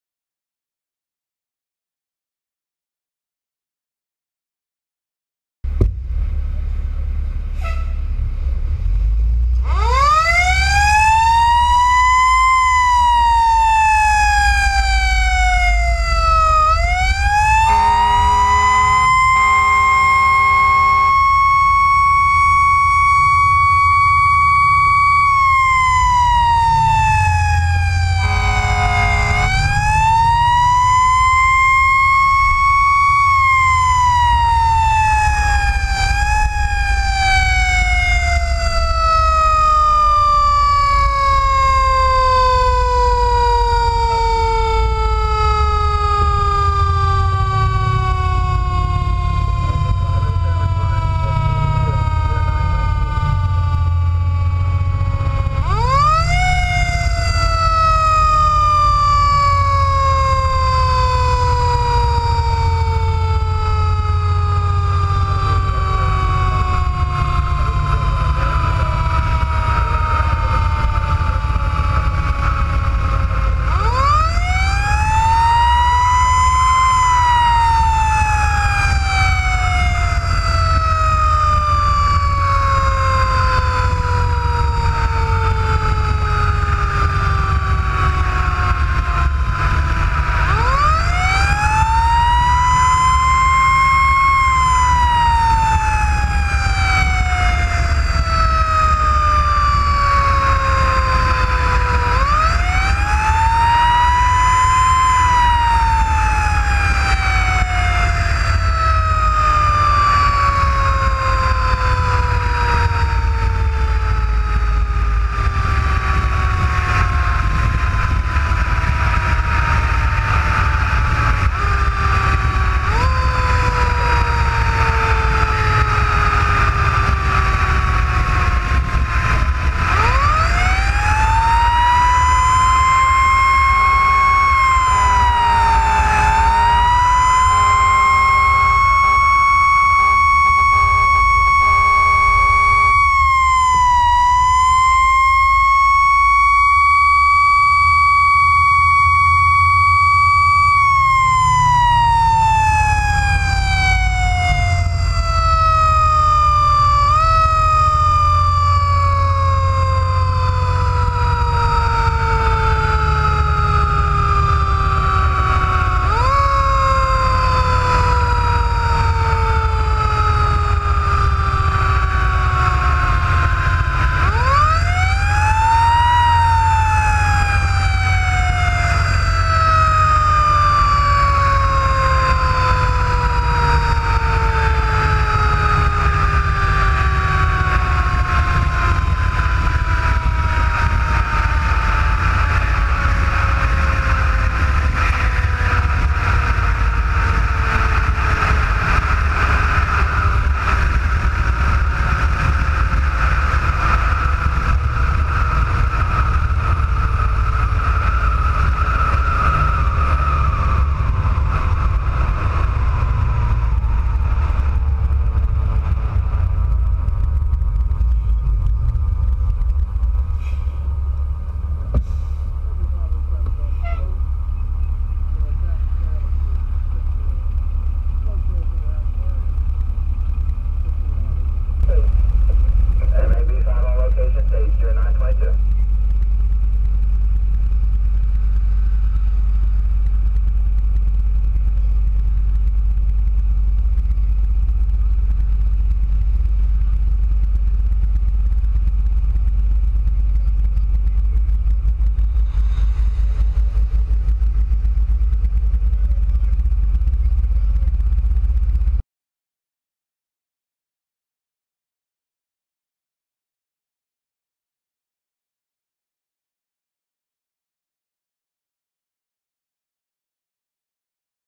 Hebron Engine 507 responds to reported vehicle fire
Hebron Fire Dept Engine 507 responded to a reported vehicle fire on eastbound Ocean Gateway. Engine 7 arrived to find no fire but an overheated wheel on a trailer was smoking.